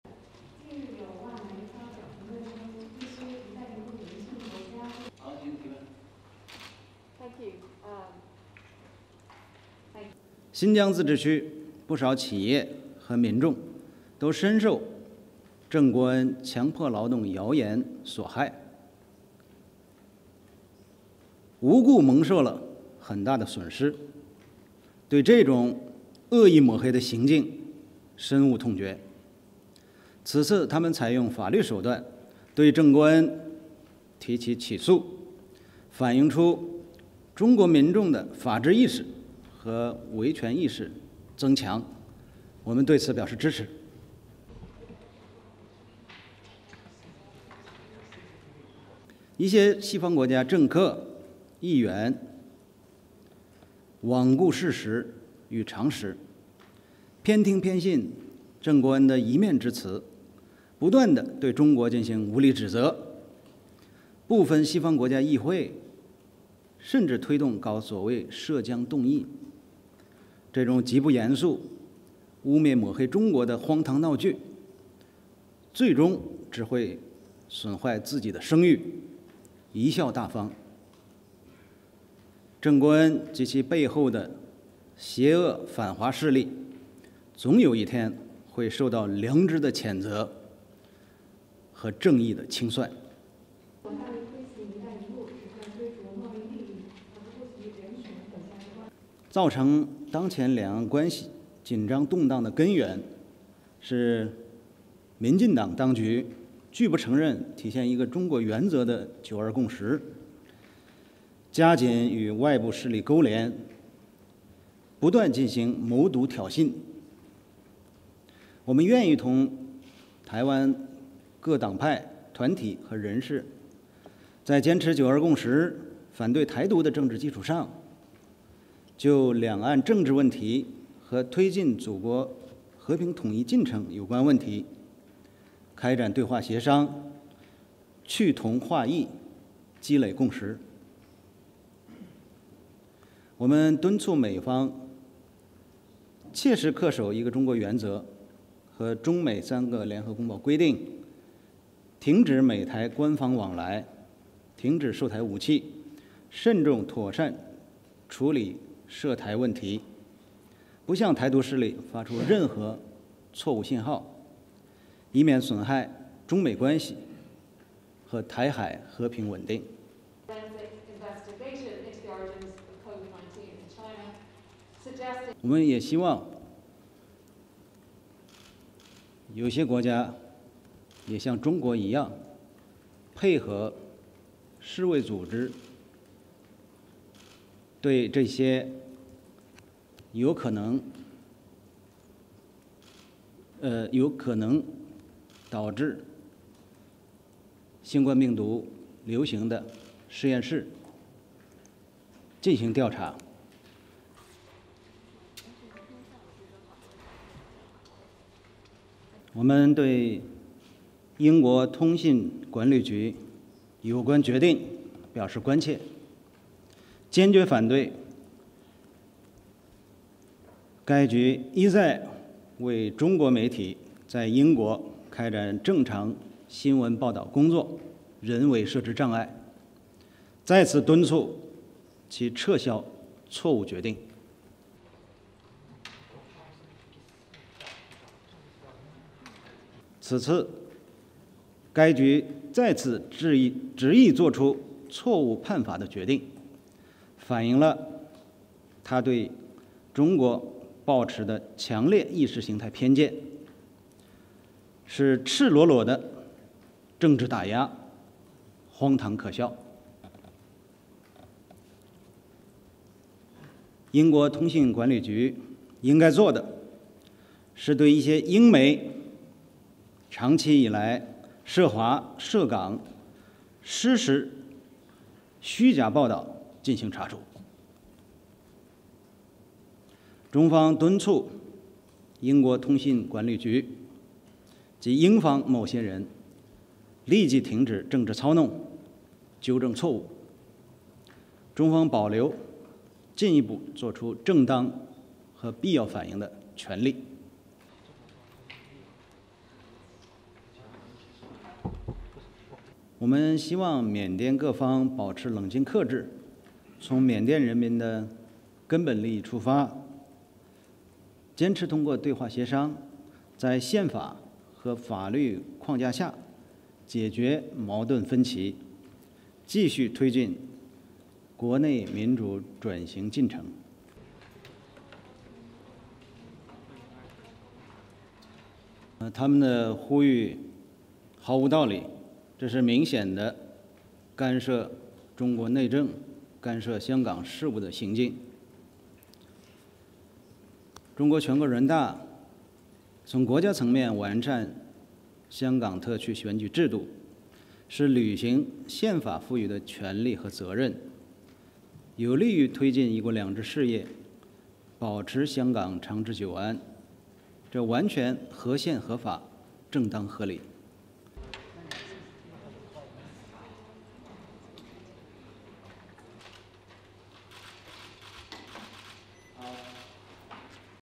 DAILY MOFA BRIEFING
(9 Mar 2021) MOFA Briefing
Spokesperson: Zhao Lijian
Date: 9 March 2021
Duration: 06:10